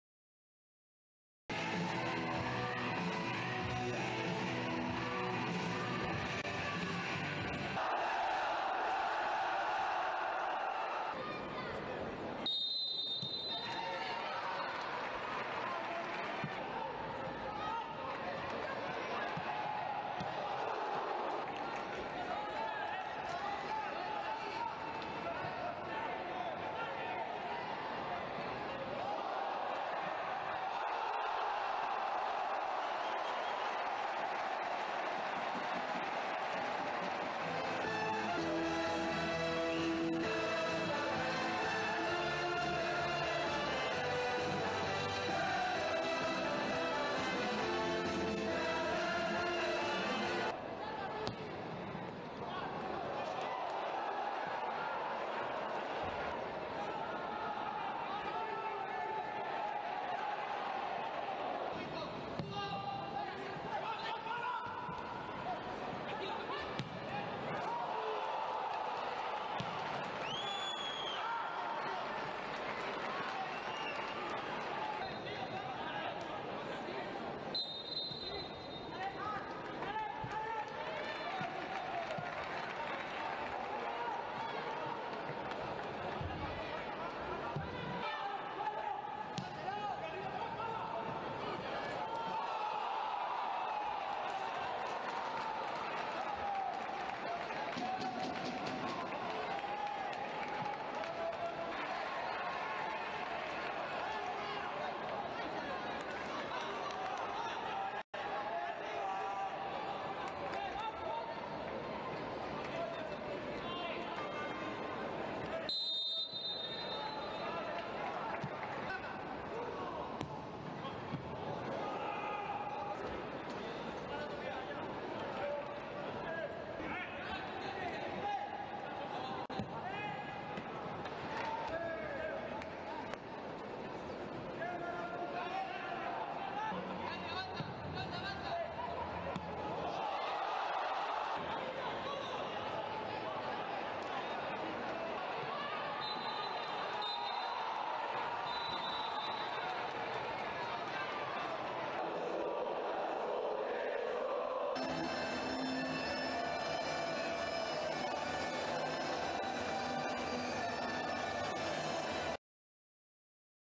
RC Celta B - Real Madrid Castilla (1-2) | Resumen y goles | Highlights Primera Federación
El Celta B cayó derrotado (1-2) en Abanca Balaídos ante el Real Madrid Castilla en el partido correspondiente a la jornada 10 en el Grupo I de Primera Federación 22/23. Miguel Rodríguez anotó el gol de los celestes.